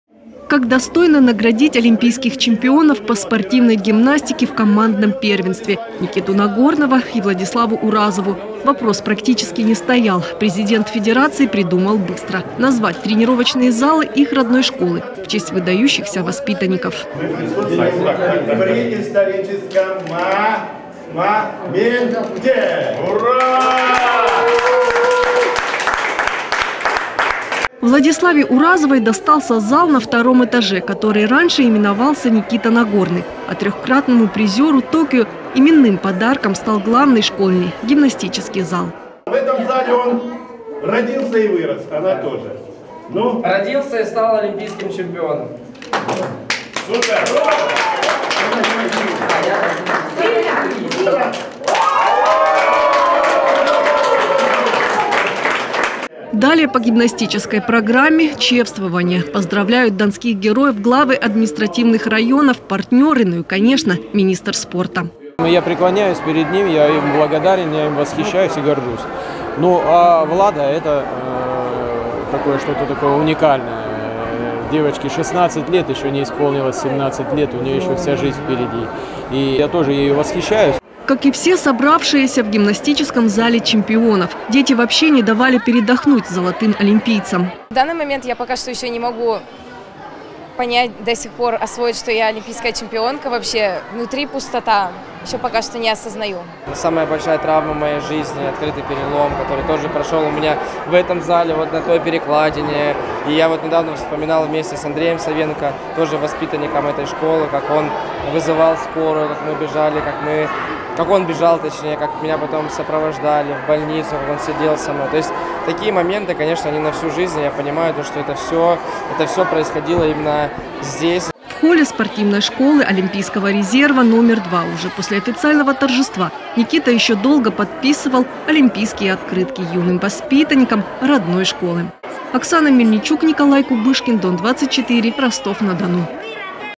Чемпионы Токио вернулись домой
Как достойно наградить олимпийских чемпионов по спортивной гимнастике в командном первенстве Никиту Нагорного и Владиславу Уразову - вопрос практически не стоял, президент федерации придумал быстро. Назвать тренировочные залы их родной школы в честь выдающихся воспитанников.

"-Мы присутствуем при историческом моменте урааа, аплодисменты"

Владиславе Уразовой достался зал на втором этаже, который раньше именовался Никита Нагорный, а трехкратному призеру Токио именным подарком стал главный школьный гимнастический зал.

"-В этом зале он родился и вырос, родился и стал олимпийским чемпионом"

Далее по гимнастической программе - чествование. Поздравляют донских героев главы административных районов, партнеры, ну и конечно - министр спорта.

Самвел Аракелян, министр по физической культуре и спорту РФ:
"Я преклоняюсь перед ним, я им восхищаюсь, горжусь. Ну а Влада это что-то уникальное Девочке 16, еще нет 17 у нее еще вся жизнь впереди. Я тоже ей восхищаюсь"

Как и все собравшиеся в гимнастическом зале чемпионов. Дети вообще не давали передохнуть золотым олимпийцам

Владислава Уразова, Олимпийская чемпионка по спортивной гимнастике:
"В данный момент я пока не могу освоить, что я олимпийская чемпионка, внутри пустота, пока не осознаю"

А Никита Нагорный рассказал нам о очень больших сложностях, с которыми столкнулся в родном зале.

Никита Нагорный, Олимпийский чемпион по спортивной гимнастике:
"Самая большая травма в моей жизни - открытый перелом, который прошел у меня в этом зале, вот на той перекладине, я недавно вспоминал вместе с Адреем Совенко , воспитанником этой школы, как он вызывал скорую, мы бежали, как он бежал - меня сопровождали в больницу, как он сидел со мной, то есть такие моменты они на всю жизнь. Это все происходило именно здесь"

В холле СШОР №2 , уже после официального торжества, Никита еще долго подписывал олимпийские открытки юным воспитанникам родной школы.